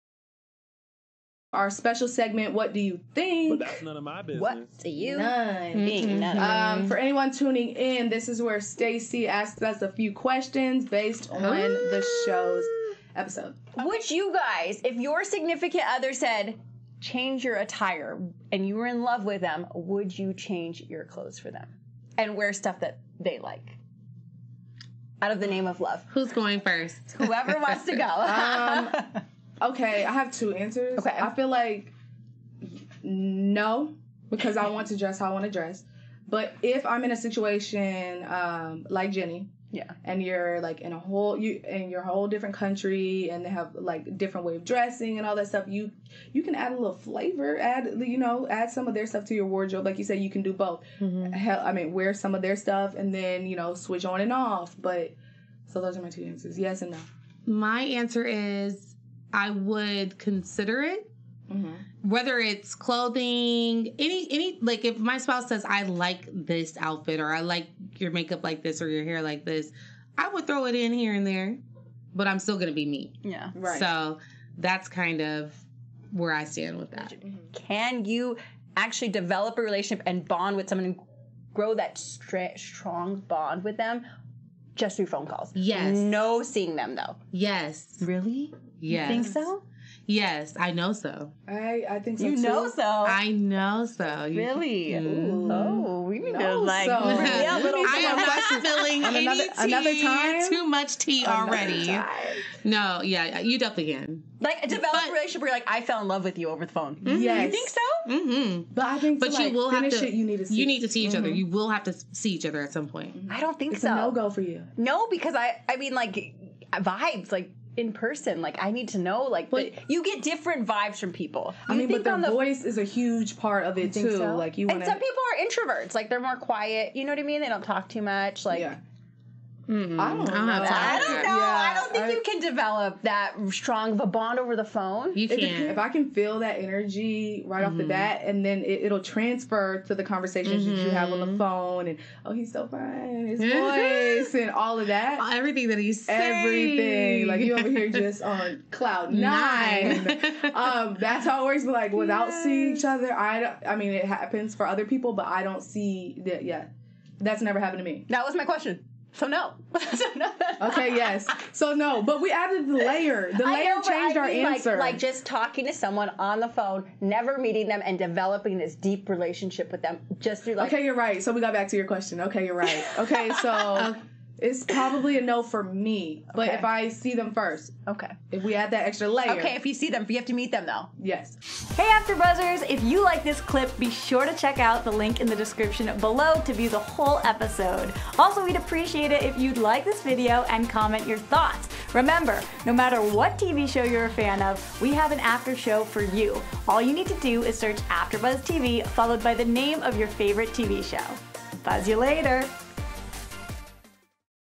90 Day Fiancé: The Other Way - What Would You Do?? (S1, E5)
Our hosts let us know what they would do if they were in the same position as the people on 90 Day Fiance: The Other Way season 1 episode 5!